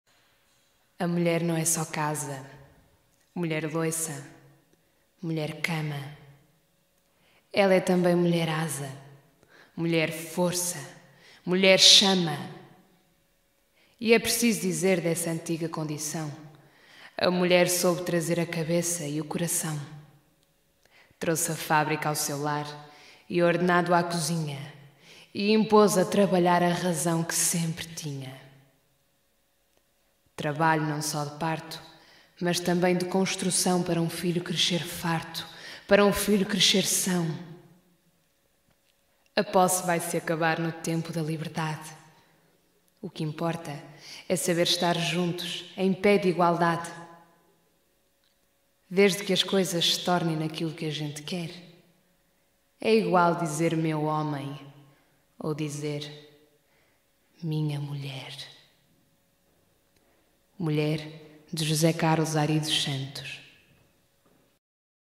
Mulher de José Carlos Ary dos Santos - Rita Calatré
Dia Mundial da Mulher - 2015
Poema de José Carlos Ary dos Santos, na voz de Rita Calatré
Edição e Gravação de Fred Meireles